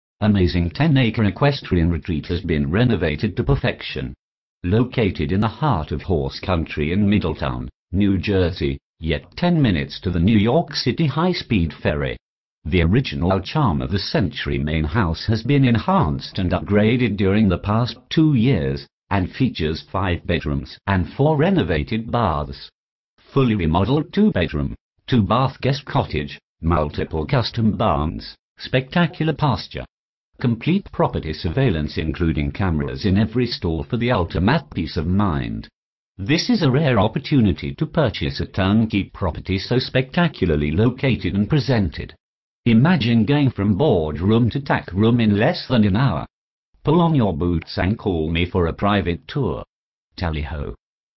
10 + Acre Equestrian Retreat | Ellen Kelly
Amazing ten acre equestrian retreat has been renovated to perfection.  Located in the heart of horse country in Middletown, New Jersey , yet  ten minutes to the New York City high speed ferry. The original charm of the century main house has been enhanced and upgraded during the past two years and features five bedrooms and four renovated baths.  Fully remodeled two bedroom, two bath guest cottage, multiple custom barns, spectacular pasture.  Complete property surveillance including cameras in every stall for the ultimate peace of mind.  This is a rare opportunity to purchase a turn-key property so spectacularly located and presented.  Imagine going from board room to tack room in less than an hour !  Tally Ho !